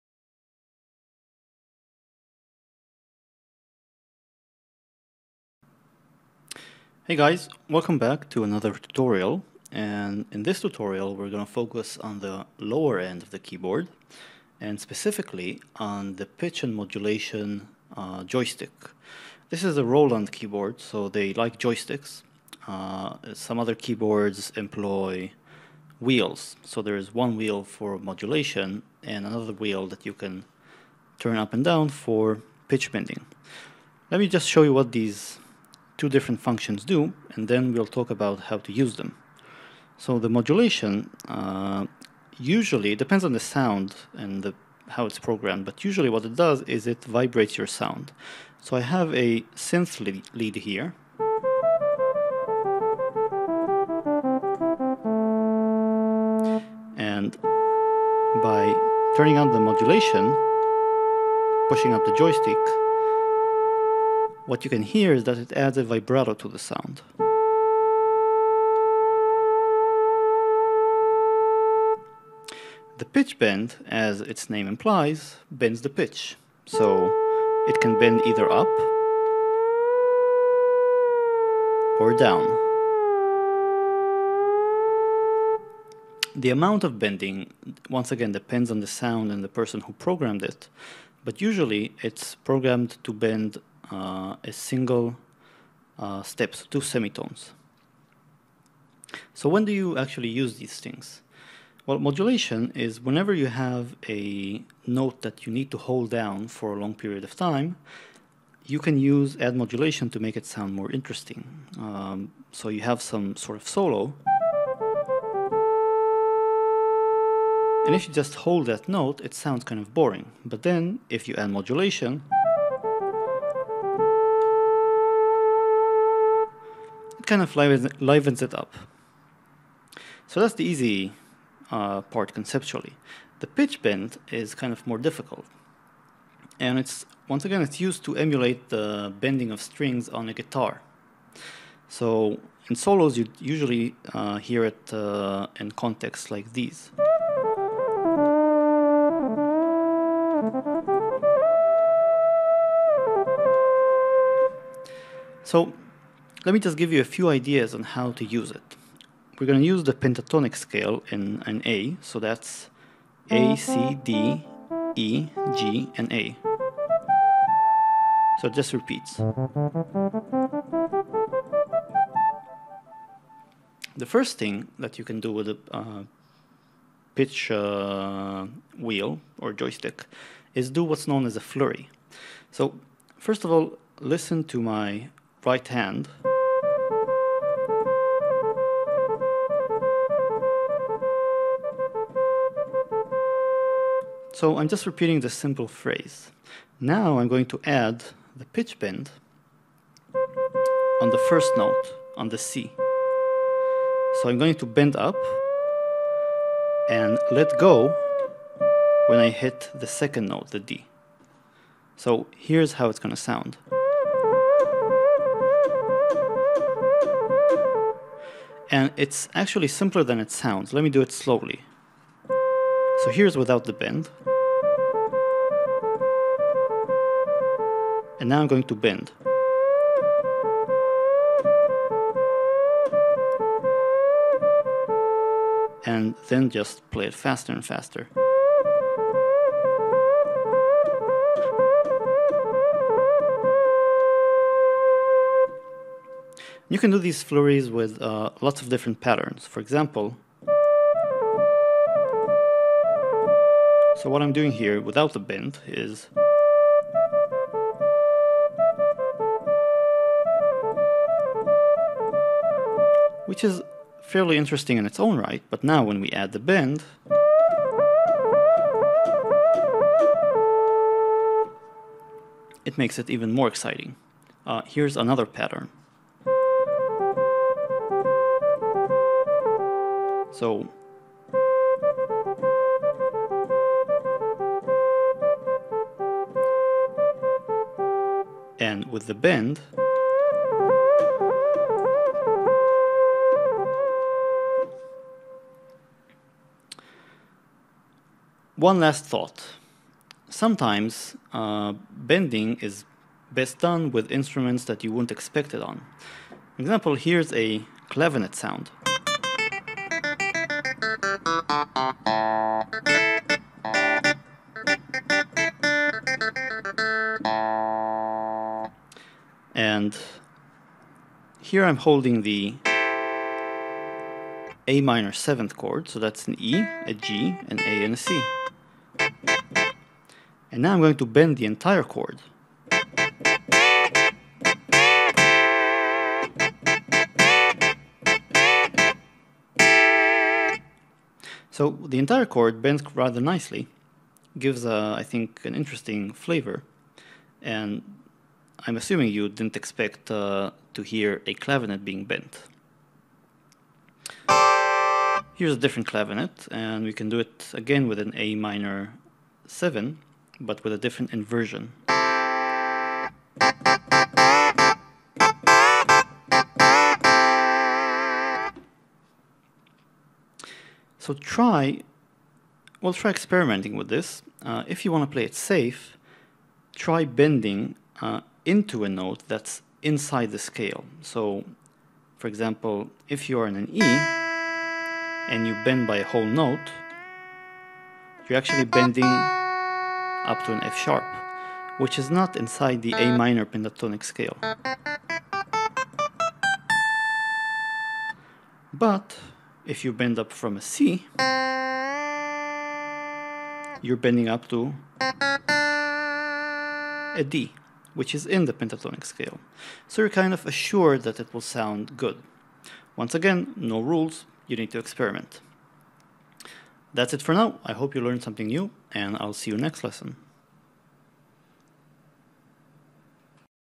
Pitch and Modulation Wheels Lesson
Learn how to enhance your synthesizer solos with the pitch and modulation wheels (or joystick) found on almost any synthesizer.